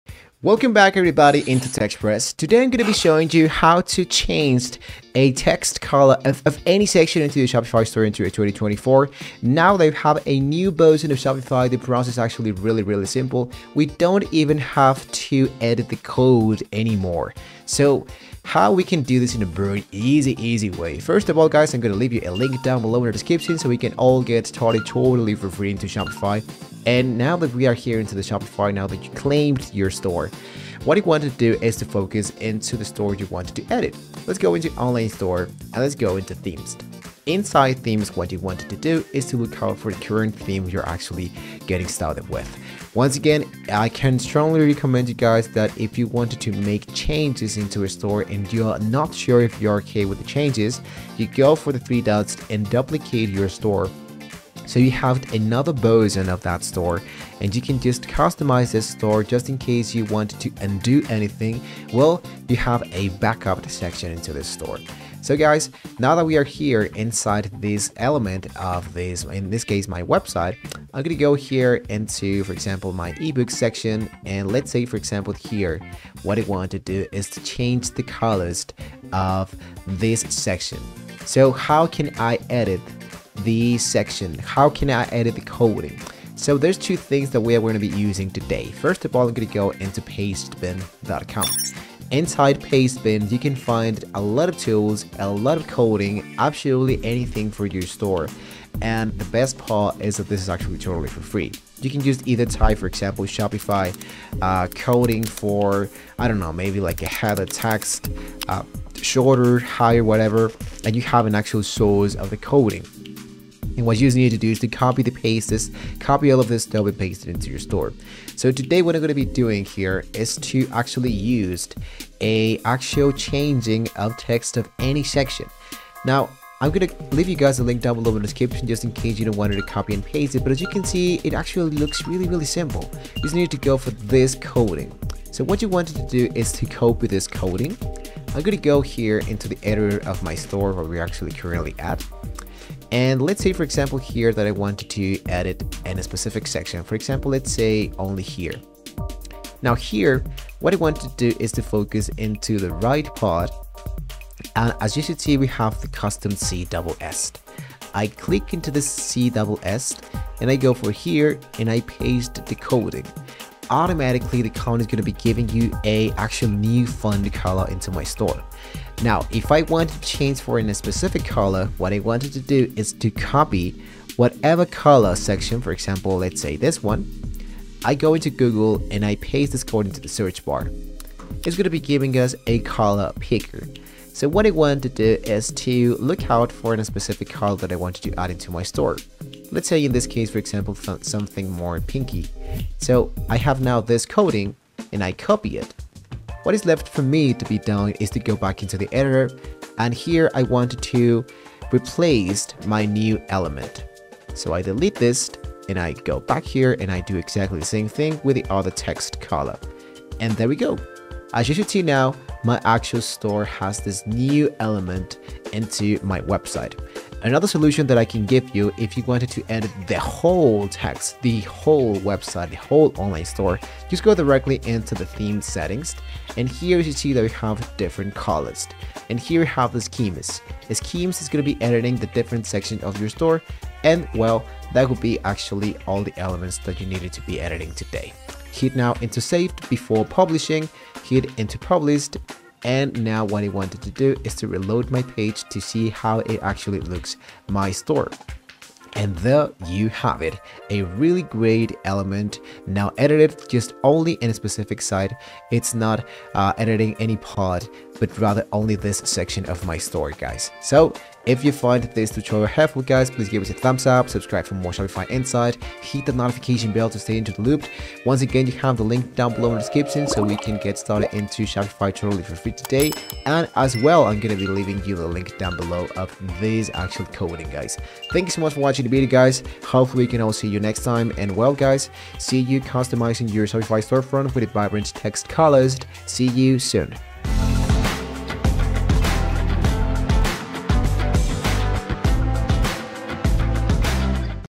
How To Change Text Color On Any Section In Shopify - Full Guide
Changing the text color on any section in Shopify is a simple and effective way to enhance the visual appeal of your online store. To begin, navigate to the "Themes" section in your Shopify dashboard and click on "Customize." From there, select the section where you want to change the text color and click on the "Edit" button. In the "Theme settings" tab, scroll down to the "Colors" section and click on the color swatch next to "Text."

A color picker will appear, allowing you to choose a new color for the text. Once you have made your selection, click "Save" and your changes will be applied instantly. With this quick and easy method, you can effortlessly customize the text color on any section in your Shopify store.